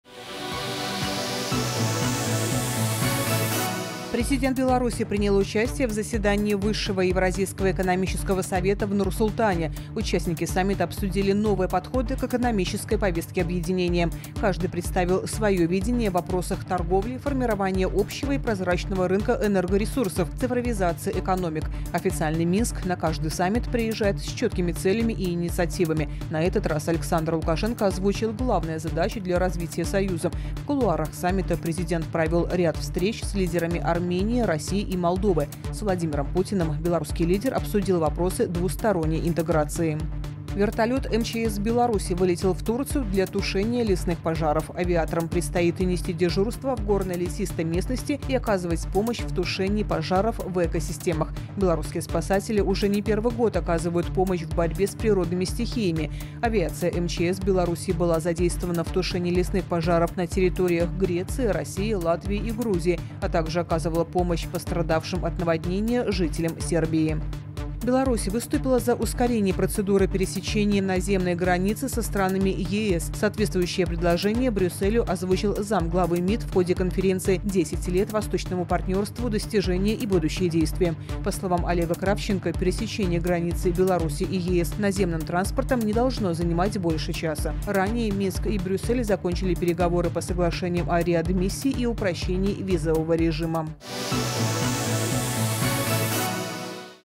Новости Беларуси 29 мая 2019, 16:00
Президент Беларуси принял участие в саммите ЕАЭС в Казахстане
Вертолет МЧС Беларуси вылетел в Турцию для тушения лесных пожаров
Беларусь выступила за ускорение процедуры пересечения наземной границы со странами ЕС